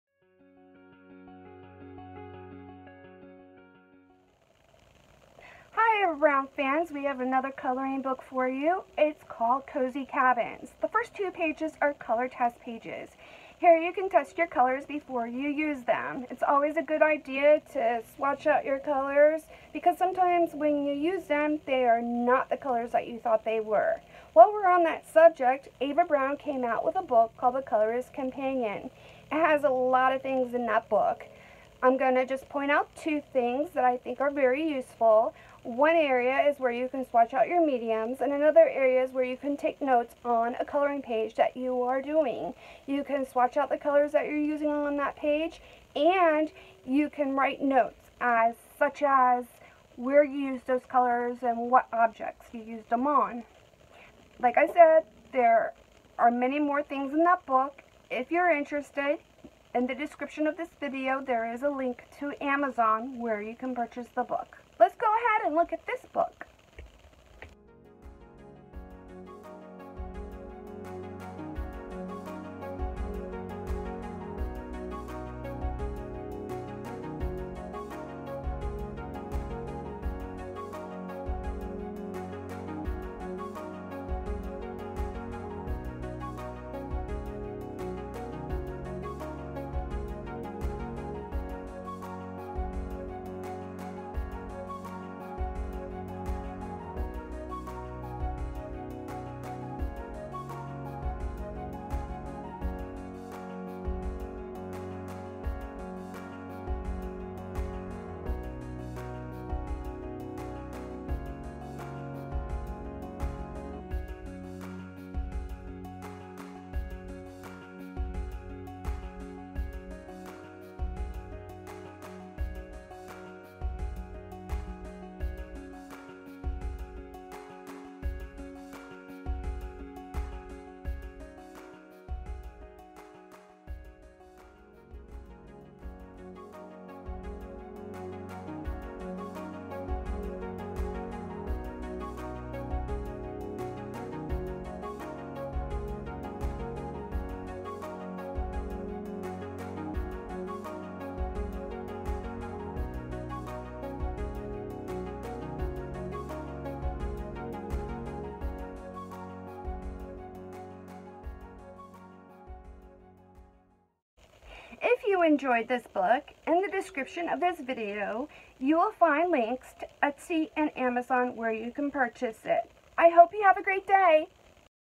Ava Browne's Cozy Cabins Coloring Book Flip Through Video
This is a flip through of Ava Browne’s Cozy Cabins Coloring Book.

If you would like to purchase this book the Amazon and Etsy links are:

If you are interested in the Colorist’s Companion (as mentioned in the video) you can purchase it through Amazon.
The Amazon link is